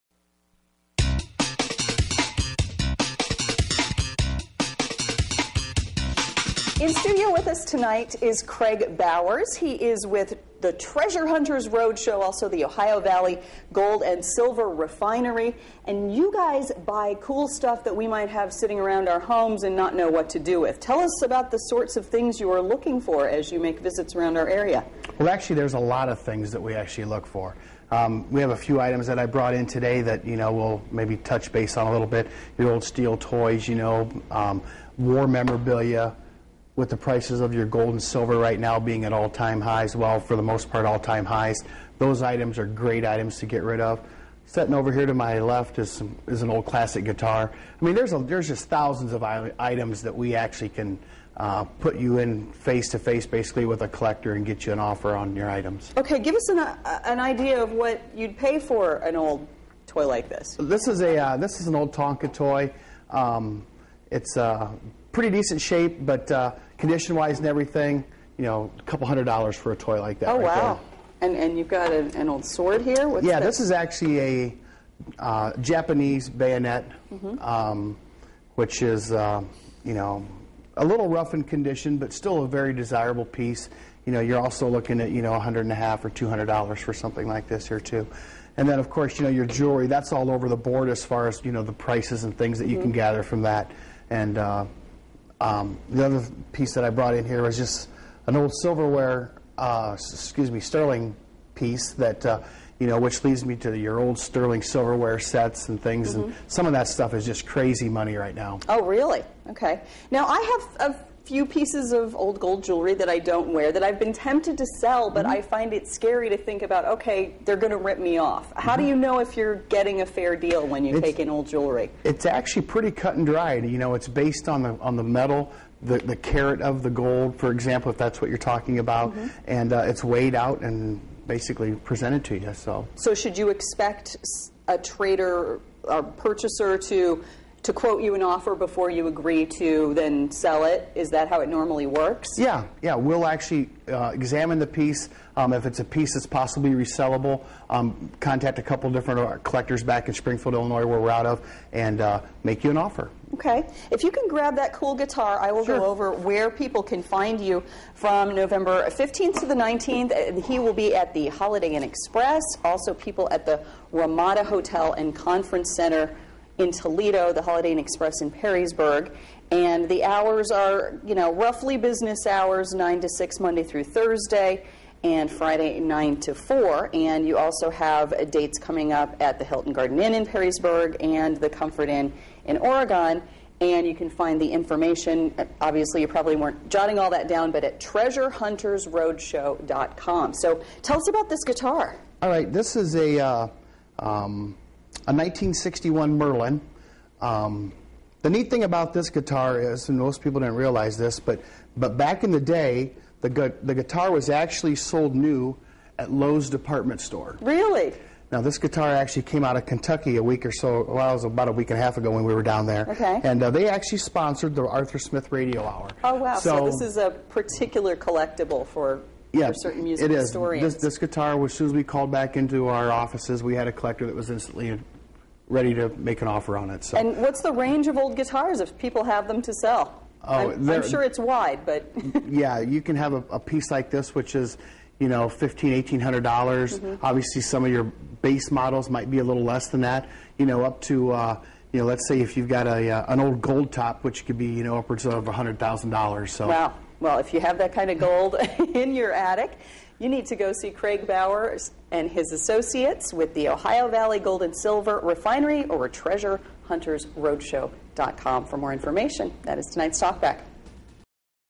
Treasure Hunters Road Show preview Nov 15-19 2010
Got gold to sell, or cleaning other valuables out of the attic?

Craig Bowers with Ohio Valley Gold and Silver Refinery, makes it his business to buy precious metals and collectibles. He talks about the Treasure Hunters Road Show, appearing at the Hampton Inn, in Monroe, Mich., Nov. 15-19.